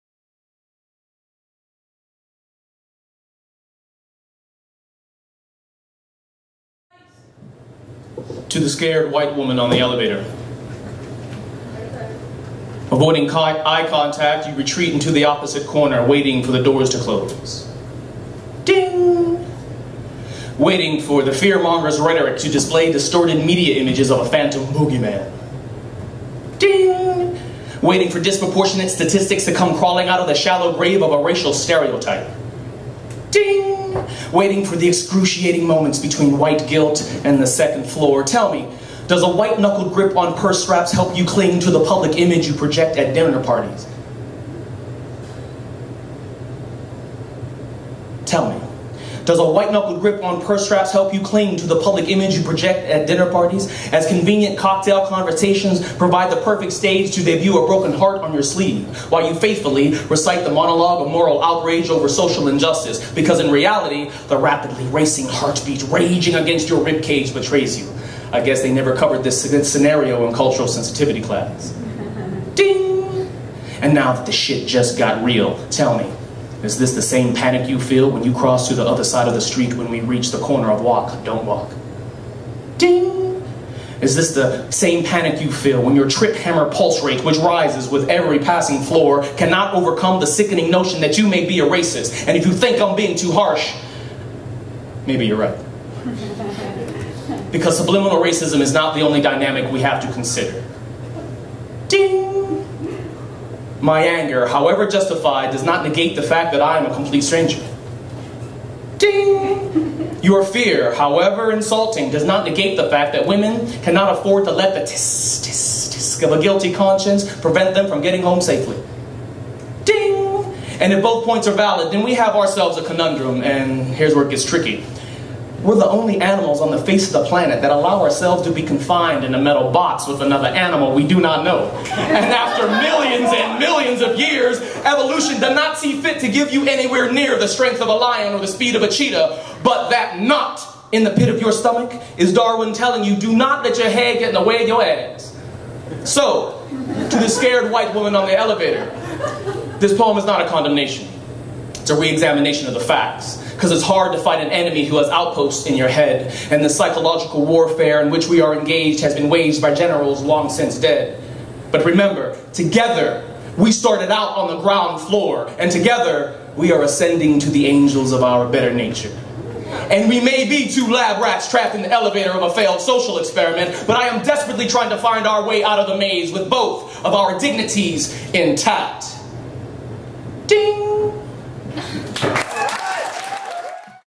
"The Elevator Conundrum" by Fannon Holland
Poem performed by Fannon Holland at a Guelph Spoken Word poetry slam (19 Sep 13 @ eBar in Guelph)